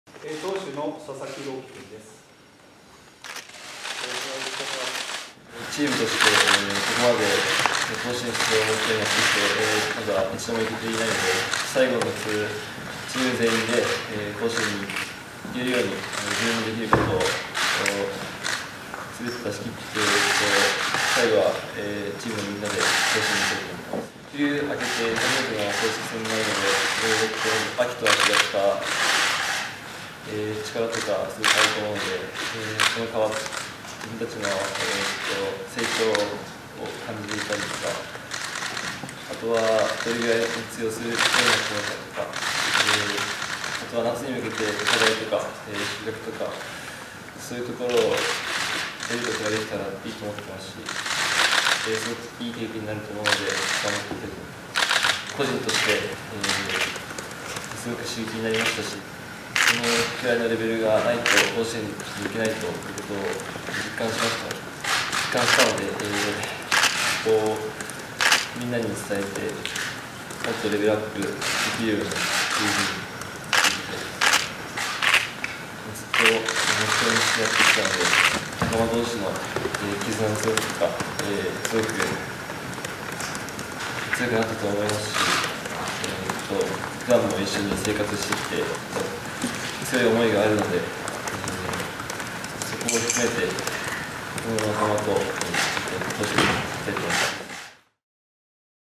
高校野球史上最速163㌔ 大船渡・佐々木が背負う思い
高校野球史上最速となる１６３㌔を計測した岩手・大船渡高の佐々木朗希投手（３年）らが４月２９日、春季岩手県大会を前に大船渡市内で記者会見に臨んだ。最後の夏に向けたシーズンの本格的な幕開け。佐々木は「この仲間で甲子園に行けるよう、自分のできることをすべて出したい」と語った。
　佐々木は１９０センチの長身右腕。高校日本代表の研修合宿の紅白戦では、プロ野球中日スカウトのスピードガンで自己最速を６キロ更新する１６３キロをマークした。大谷翔平が花巻東（岩手）時代に記録した高校生最速の１６０キロを上回った。
　高い関心と注目を集めていることから、県大会を前に特別に会見の場が設けられ、千葉宗幸主将、及川恵介捕手、国保陽平監督も出席した。約４０人の報道陣が詰めかけた。昨秋の県大会は準決勝で盛岡大付に、３位決定戦でも専大北上に敗れ、東北大会への出場を逃した。５月２日に初戦が予定されている春季県大会沿岸南地区予選は、それ以来となる公式戦だ。今月２０日の練習試合には日米あわせて約２０球団のスカウトが集まるなど、岩手の県立校は大きな注目を浴びながら夏へ向かっていく。